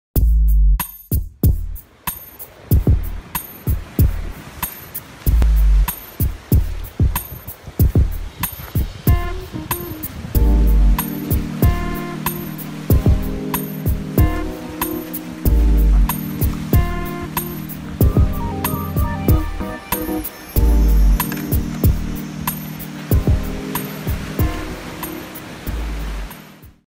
Playa Lucía, Yabucoa
La Playa Lucía está localizada en Yabucoa. Es frecuentada por personas para ver su lindo atardecer y disfrutarlo con sus amigos. Esta playa cuenta con una orilla extensa que la hace perfecta para caminar o pasar un día bajo el sol. Además es sumamente tranquila pero no para nadar debido a sus piedras, corrientes y algas que llegan a su orilla por temporadas. La Isla de Vieques se puede observar desde esta playa.